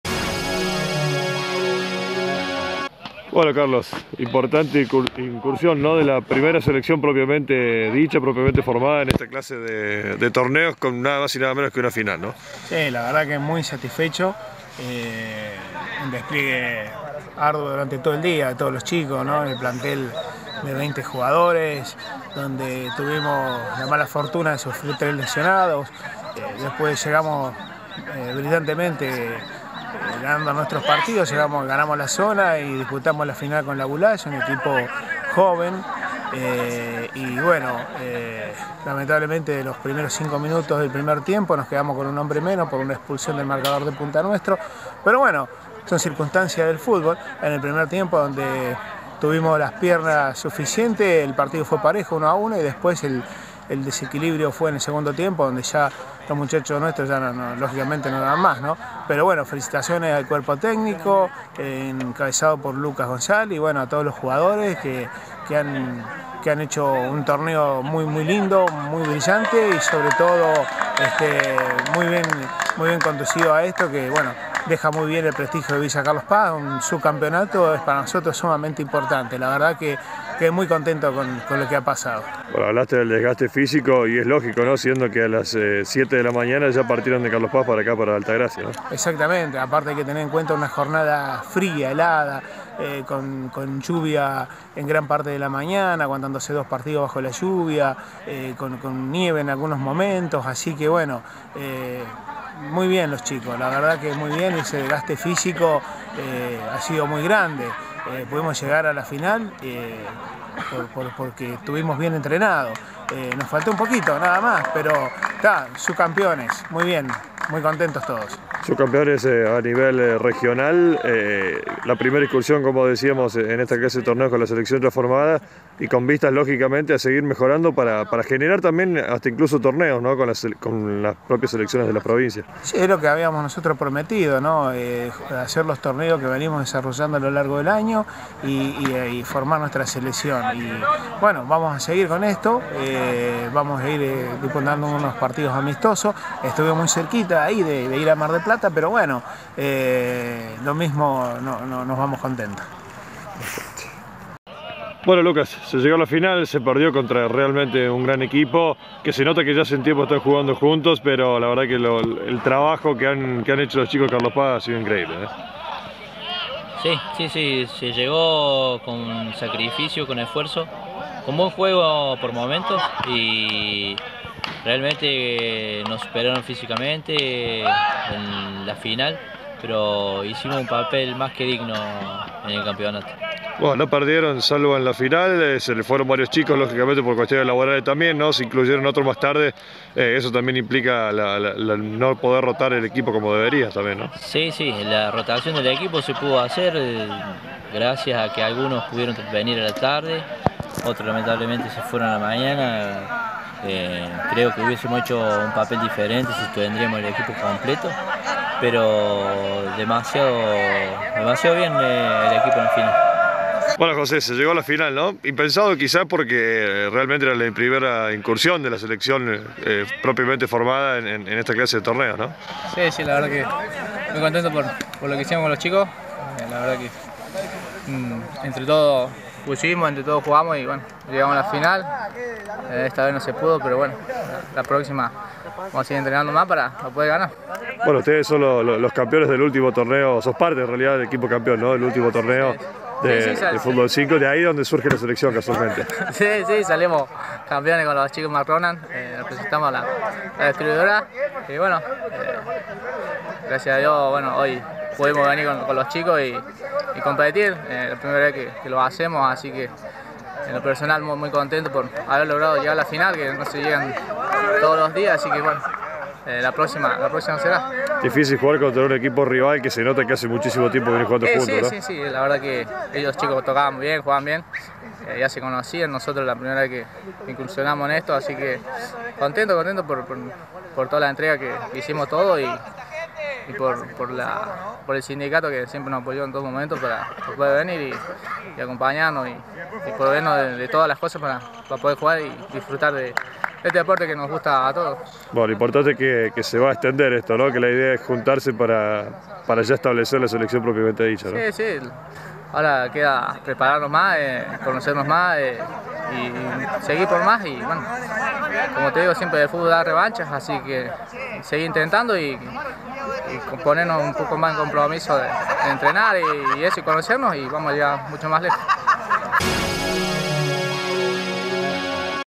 El equipo de fútbol del Centro de Empleados de Comercio subcampeón
El equipo de fútbol del Centro de Empleados de Comercio de Villa Carlos Paz salió subcampeón del campeonato provincial. El equipo de Carlos Paz se enfrentó contra el Centro Empleados De Comercio Laboulaye  pero no logró obtener el pase para disputar el Torneo Nacional de Faecys en Mar del Plata.